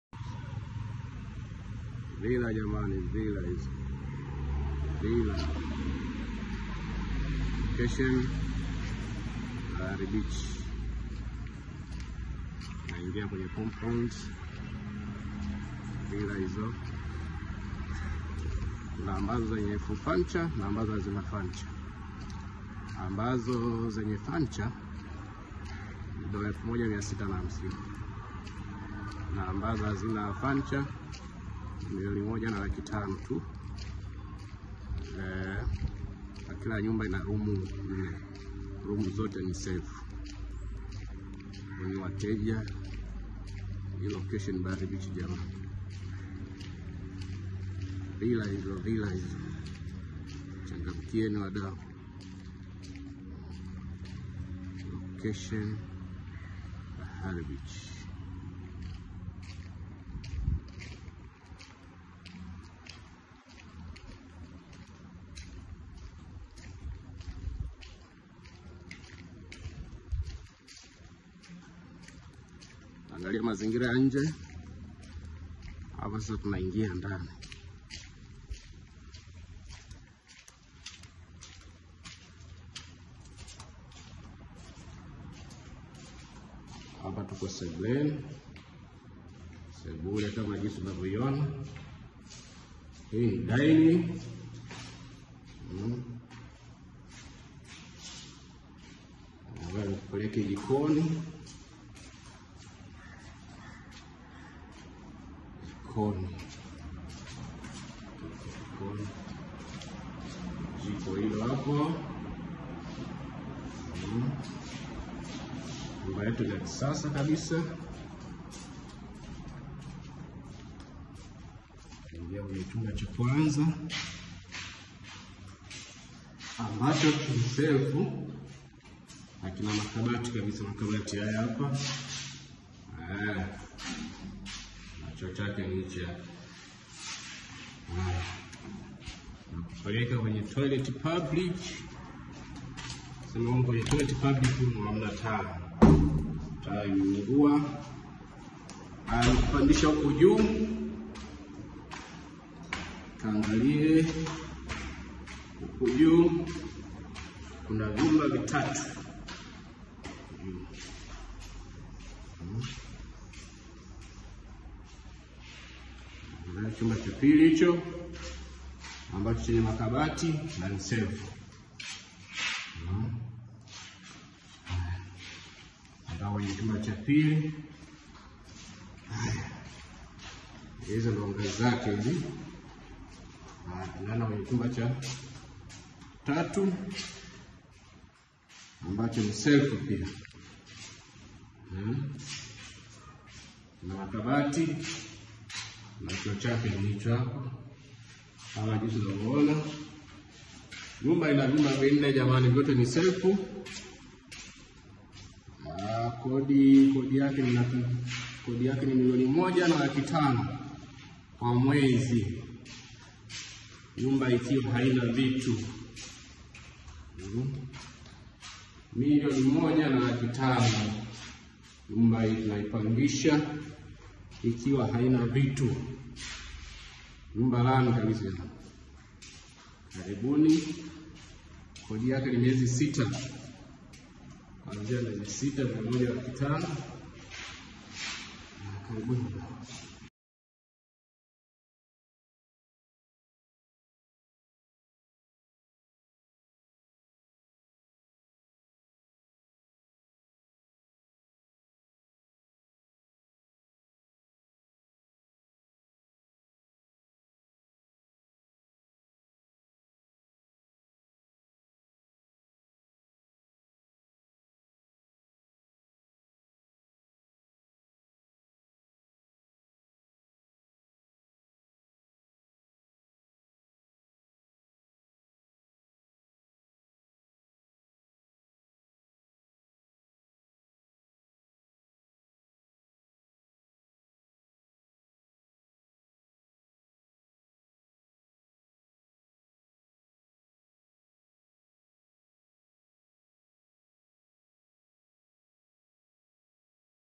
VILA APARTMENT YENYE VYUMBA VINNE INAPANGISHW MILIONI 1,500,000 KWA MWEZ IPO BAHARI BEACH 0717671240
VILLA KALI SANA INAPANGISHWA

LOCATION  BAHARI BEACH

INA VYUMBA VINNE  ,VYOTE SELF SEBULE DINING JIKO TOILET PUBLIC

FULL MAKABATI VYUMBANI NA JIKONI

FULL PEVER FULL A/C

FULL SECURITY FENCE

PRICE MIL 1.5 PER MONTH

MALIPO KUANZIA MIEZI 6